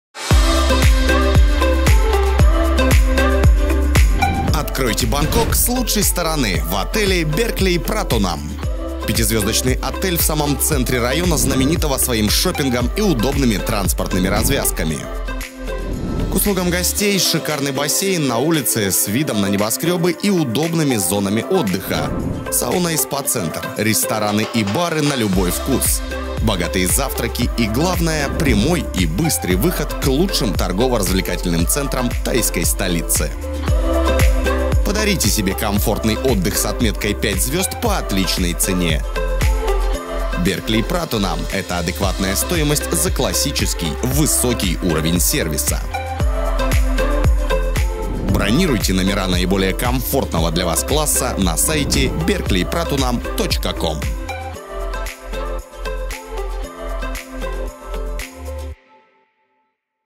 The Berkeley Hotel Pratunam
The Berkeley Hotel Pratunam. Этот 5-звездочный отель - вариант с прекрасным расположением в районе центра Бангкока. Рядом с ним находится рынок Пратунам, а в 3 минутах ходьбы - торговый центр Platinum Fashion, торговый комплекс CentralWorld находится в 0,7 км.
Посещая город Бангкок, вы почувствуете себя как дома в The Berkeley Hotel Pratunam, где вы всегда найдете отличный сервис и доброжелательную атмосферу. Мы предлагаем вам безупречное обслуживание и все необходимые составляющие для хорошего отдыха. Объект размещения предоставляет своим гостям обслуживание номеров 24/7, бесплатный Wi-Fi в номерах, охрана 24/7, ежедневная уборка, сувенирный магазин, чтобы обеспечить им максимальный комфорт и удобство.